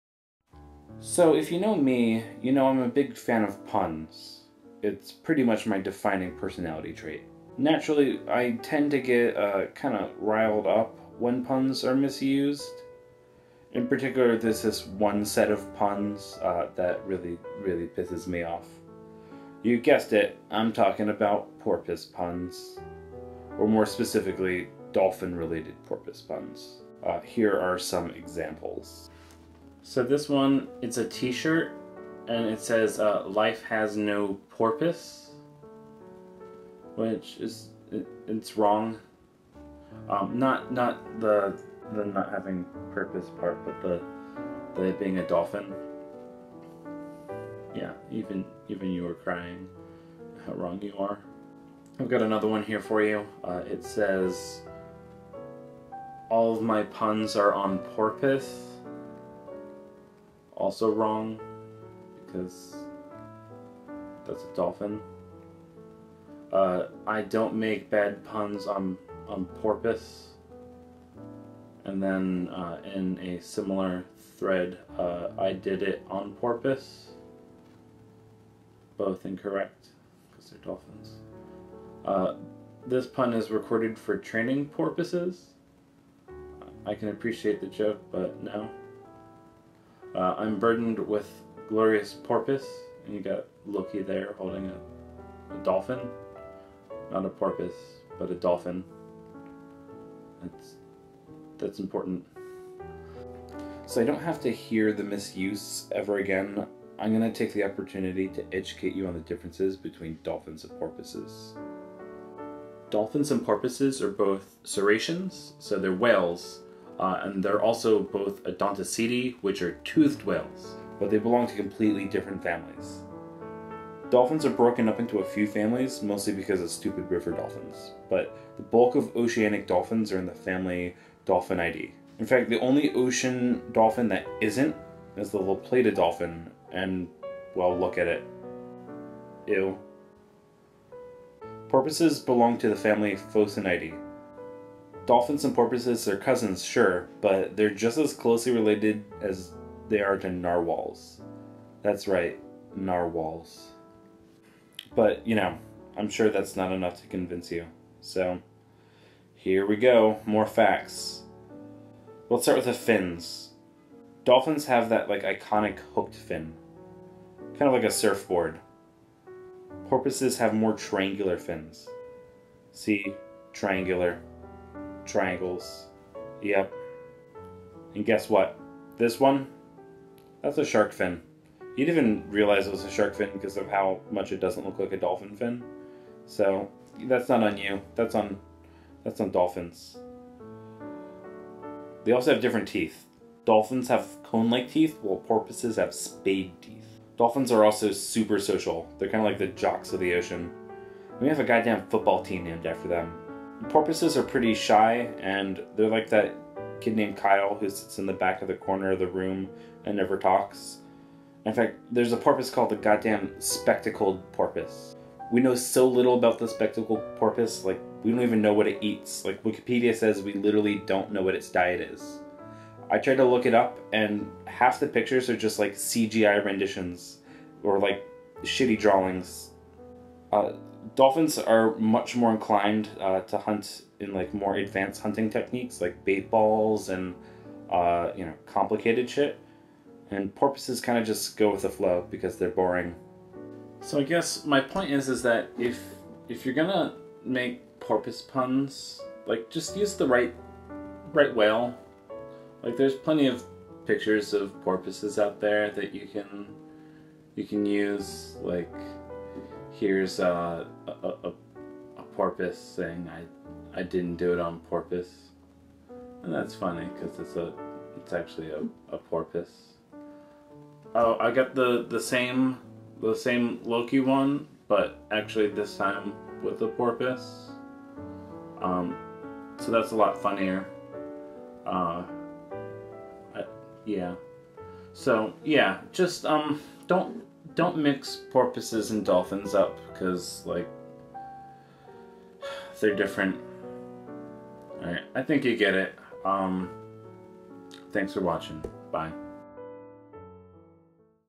A Public Service Announcement
This video is about the differences between dolphins and porpoises.